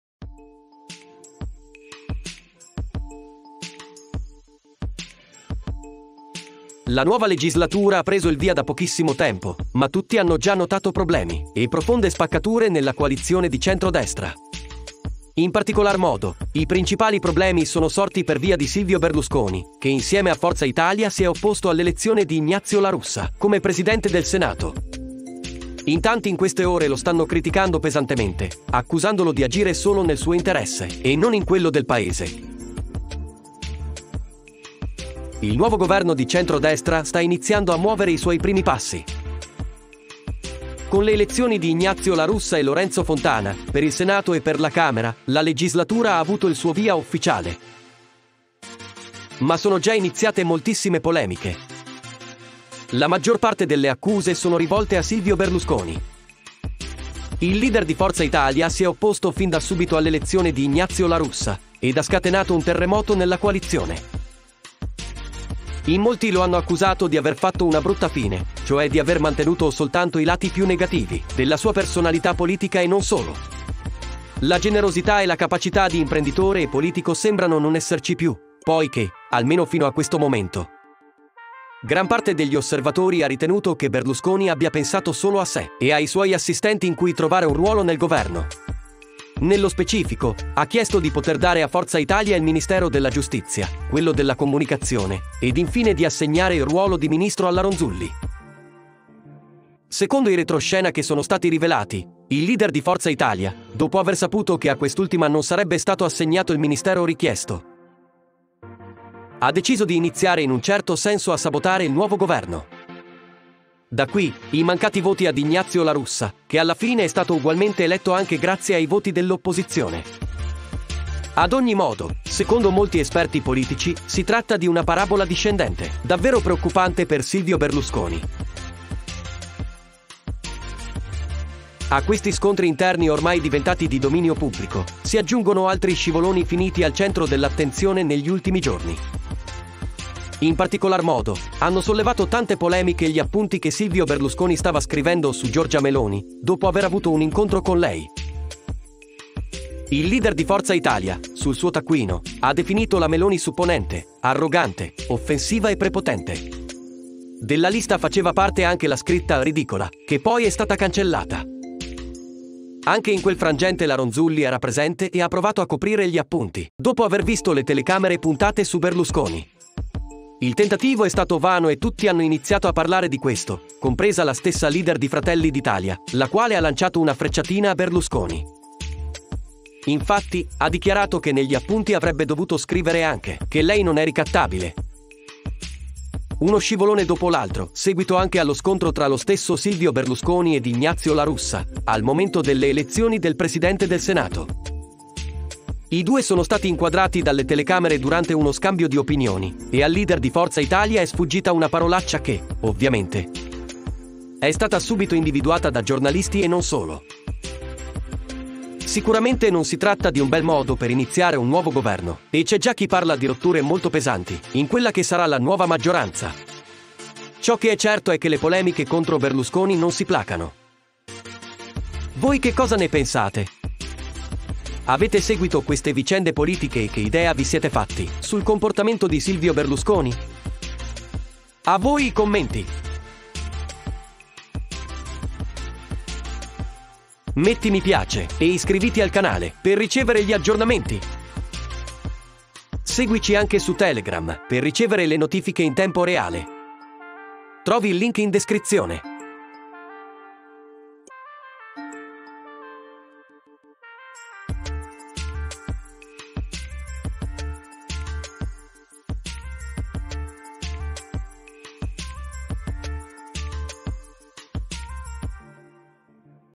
Nuovo Governo: La Brutta Figura Di Silvio Berlusconi!
Nuovo governo: cominciano già gli scontri nella coalizione ed in particolar modo Silvio Berlusconi è al centro di polemiche ed accuse, per via del suo comportamento a pochi giorni dall'inizio della legislatura...